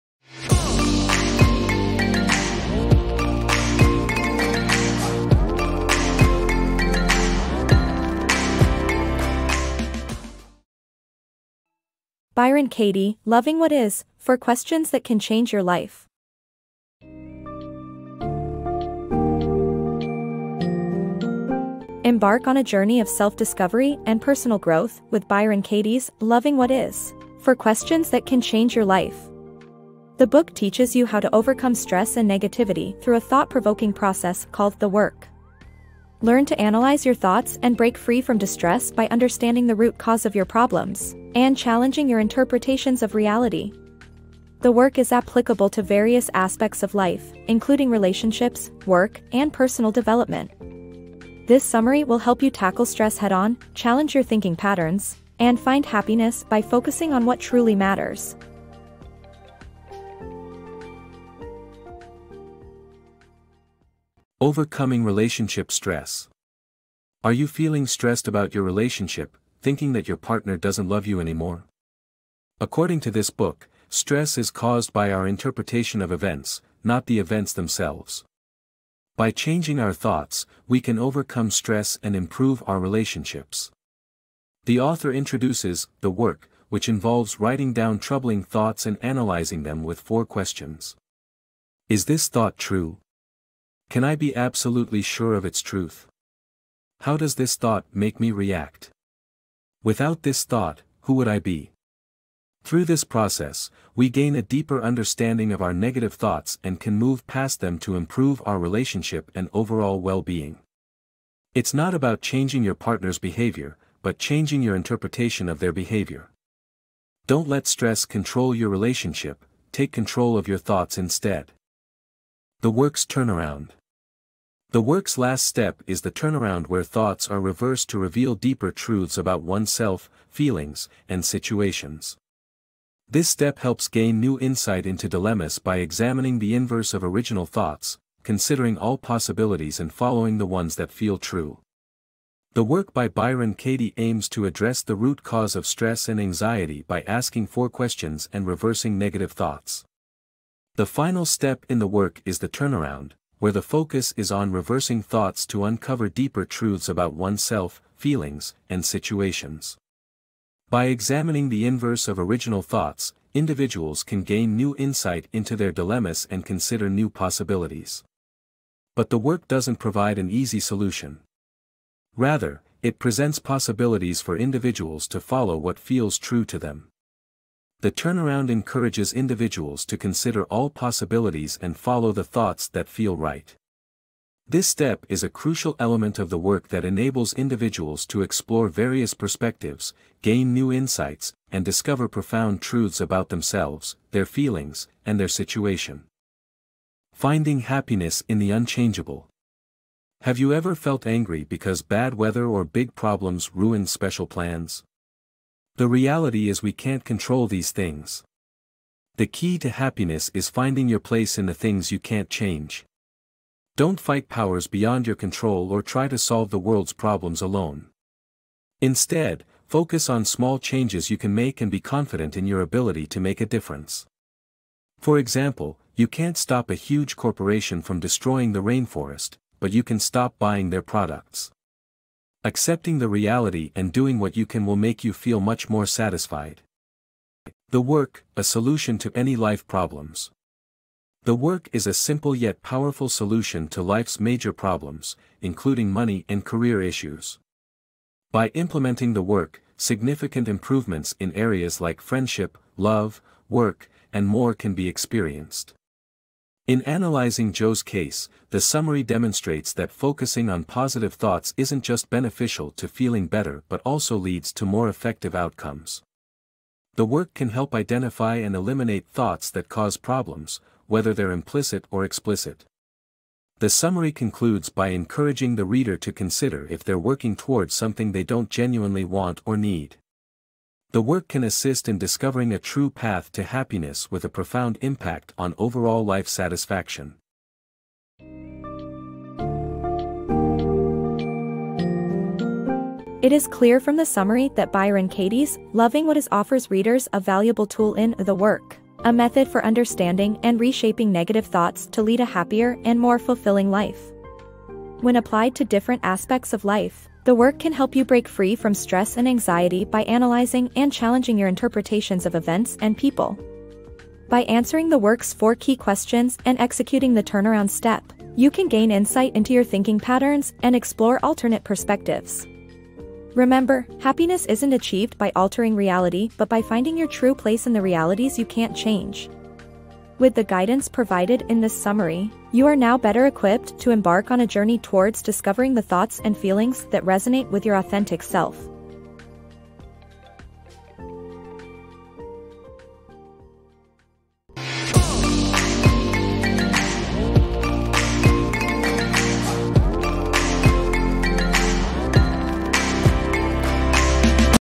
Loving What Is by Byron Katie: 5 Minute Summary
*BOOK SUMMARY*

TITLE - Loving What Is: Four Questions That Can Change Your Life

AUTHOR - Byron Katie

DESCRIPTION:
📖 Looking for an easy-to-use process to solve everyday problems? Byron Katie's Loving What Is teaches you The Work - four simple questions to challenge your thinking. Apply them to specific issues and watch as the thought process turns from negative to positive, enabling you to appreciate life exactly as it is.

TIMESTAMPS:
⌚ 00:00 Introduction
⌚ 01:05 Overcoming Relationship Stress
⌚ 02:03 The Work's Turnaround
⌚ 03:17 Finding Happiness in the Unchangeable
⌚ 03:58 The Work: A Solution to any Life Problems
⌚ 04:48 Final Recap